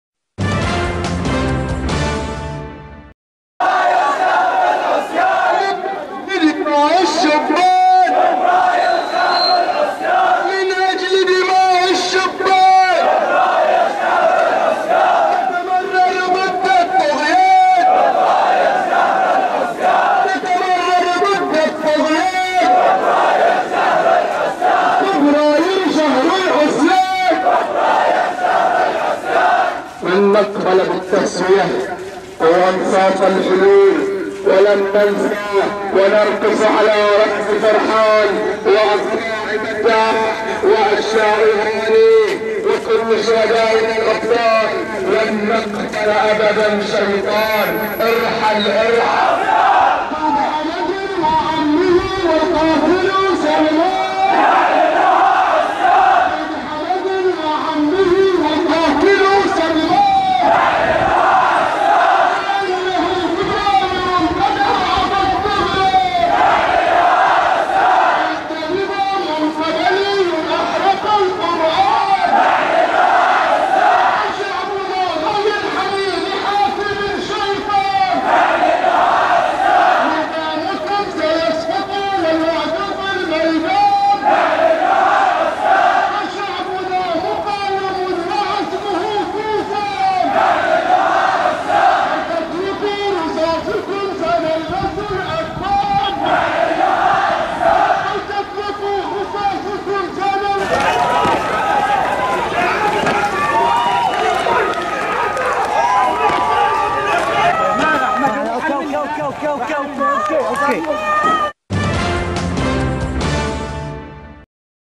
قوات النظام تقمع تظاهرة حاشدة تحت شعار "عصيان العزة" في سترة 28/1/2014 Bahrain
قمعت قوات النظام الخليفي مساء يوم الثلاثاء (28 يناير/كانون الثاني 2014) مسيرة خرجت بمنطقة واديان بجزيرة سترة تحت شعار "عصيان العزة" رفضاً لأي تسويات مع النظام الحاكم في البحرين، وتأكيدا على إسقاطه، وشارك في المسيرة حشود غفيرة حملوا أعلام البحرين ولافتات كتب عليها "لا للحوار مع القتلة"، وردد المتظاهرين شعارات مناهضة للنظام الخليفي وشعارات تدعو للإطاحة بحاكم البحرين حمد بن عيسى آل خليفة، المسيرة التي خرجت توجهت نحو مركز للشرطة سرعان ما قمعتها قوات المرتزقة بالغازات السامة ومسلات الدموع وأعيرة نارية من الرصاص الإنشطاري "الشوزن" موقعة عدد من المصابين في صفوف المتظاهرين.